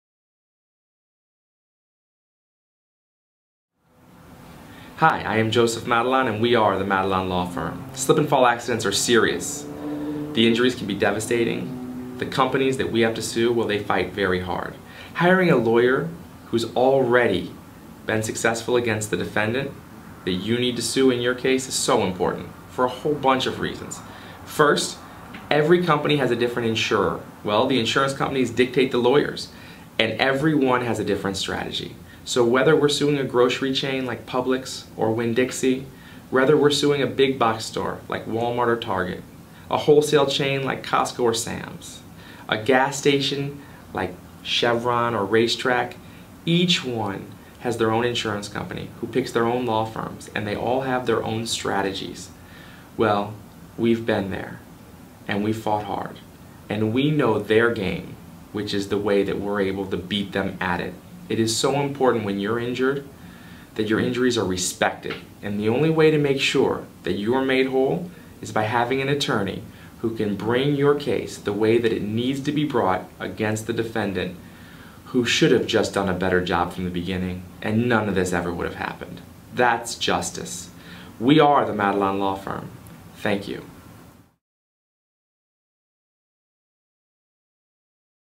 Slip and Fall Accidents - Helping you understand the law
Slip and Fall accident attorney Joseph Madalon is here to answer all your questions about your accident.

Florida law is exceptionally serious when it comes to slip-and-fall cases. You, as the victim, carry the burden of proof and may be required to compile and submit detailed evidence supporting your claim. Defendants in slip-and-fall cases will do everything they can to avoid paying damages, so your claim must be as thorough and well-supported as possible.

The Florida accident attorneys of Madalon Law are also exceptionally serious when it comes to representing victims of slip-and-fall accidents. We can help you navigate the court system and develop a concise argument.

If you've been injured in a slip-and-fall, there are a few crucial steps that you'll need to take. Whether or not you follow these steps can ultimately influence the outcome of your case.

First, take photos of everything. Often, photos are the only way to prove that there was a hazardous condition that led to your slip-and-fall.

Second, save the clothes and shoes you were wearing at the time of the accident. Do not wear or wash them again after the fall. There will be stains and substances on them that may be proof of what led you to the slip-and-fall, such as melted frozen yogurt, fruit, oil, etc.

Winning a slip-and-fall case in Florida is challenging but certainly not impossible with the support of a reliable, experienced attorney.